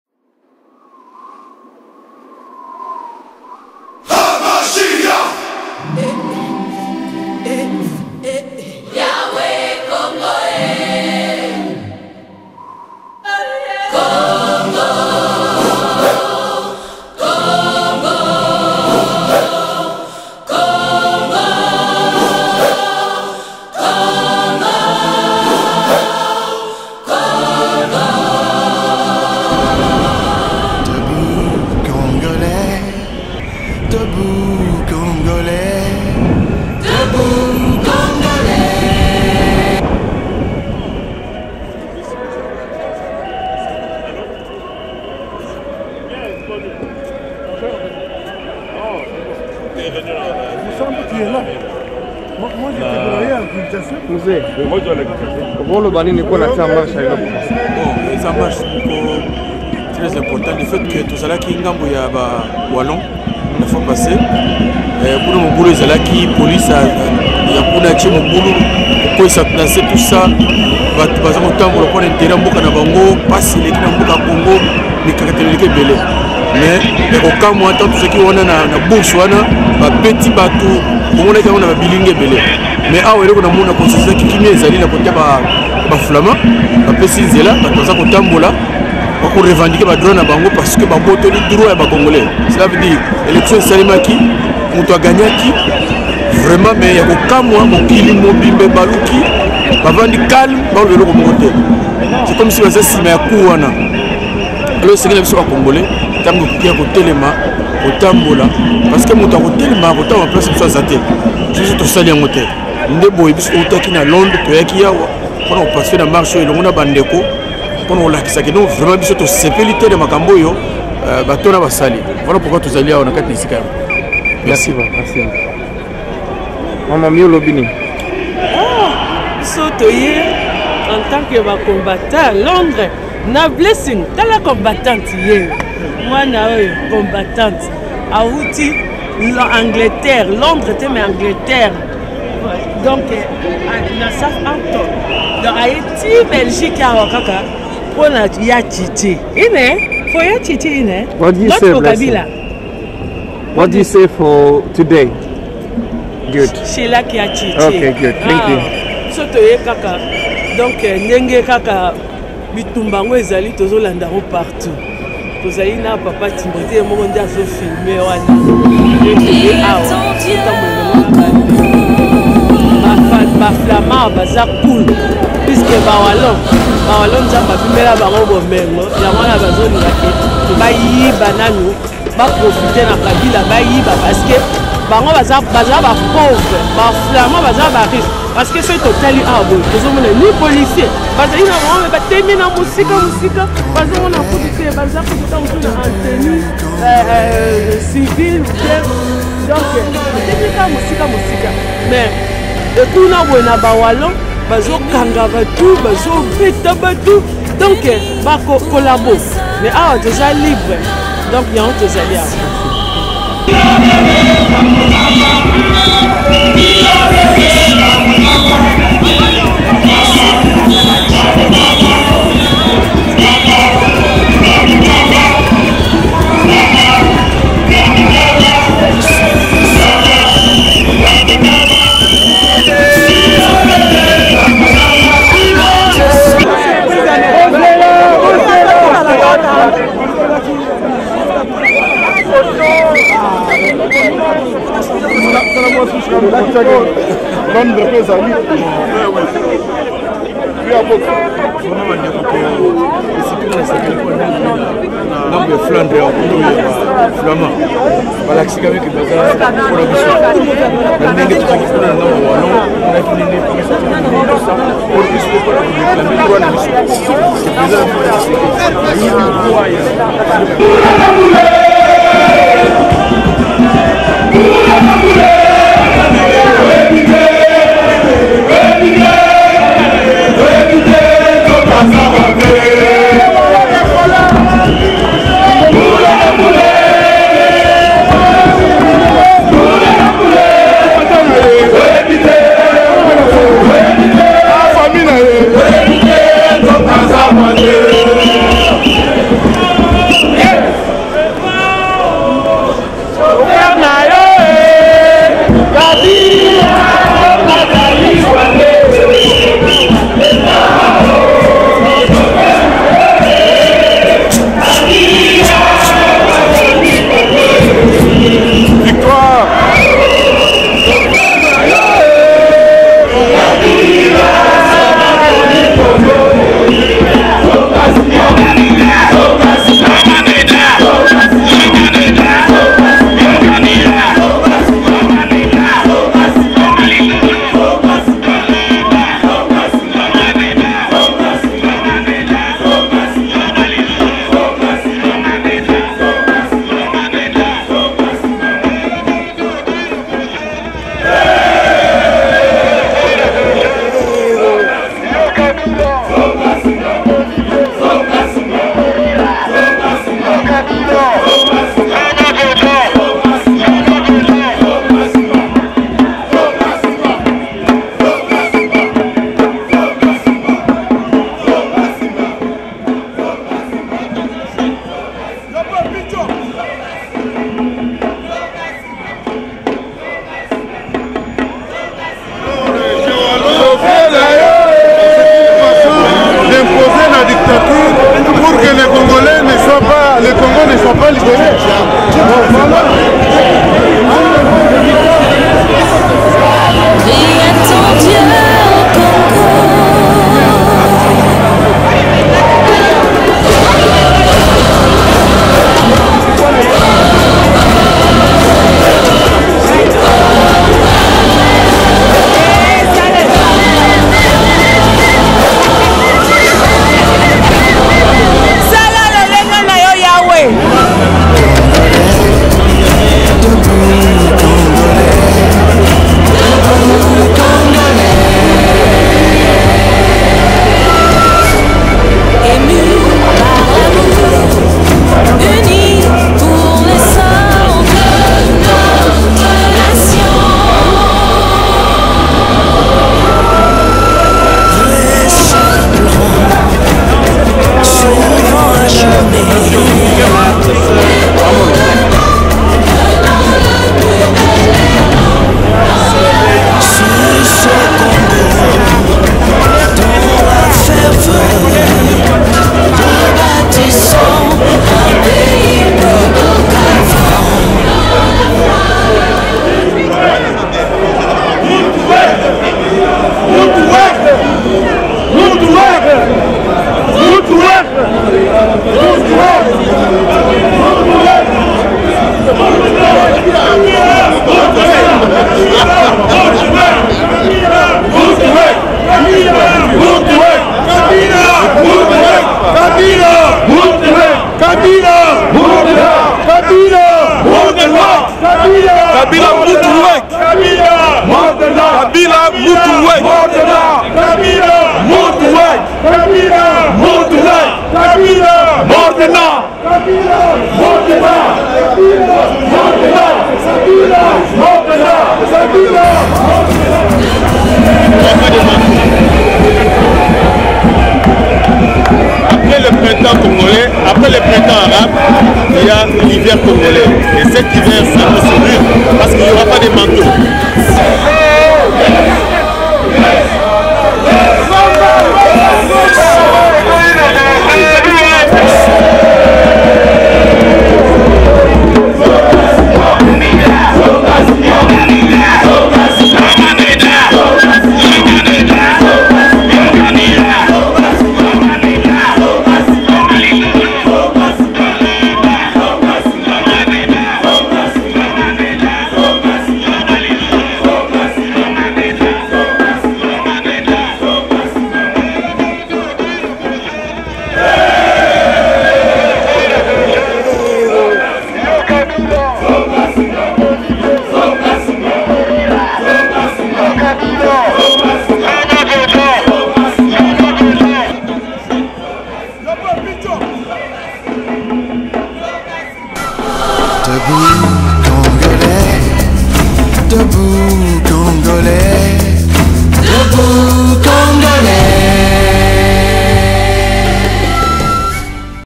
Kabila va partir, Leuven, Belgique ( Apotre Dany )
marche de protestation a Leuven Bruxelle
la communaute Congolaise en marche pour sa liberation